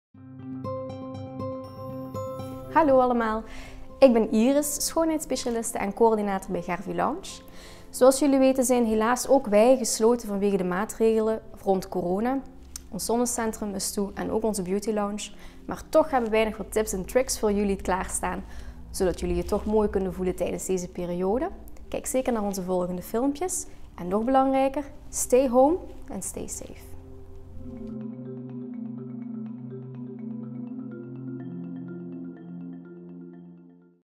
Stay home, stay beautiful met Gervi Lounge
Gervi Lounge is momenteel gesloten omwille van de strikte Corona-maatregelen. Maar dat wil niet zeggen dat jij je niet mooi wilt voelen tijdens deze periode... Daarom hebben wij enkele tips en tricks voor jou zodat je je opperbest voelt, ook tijdens een lockdown.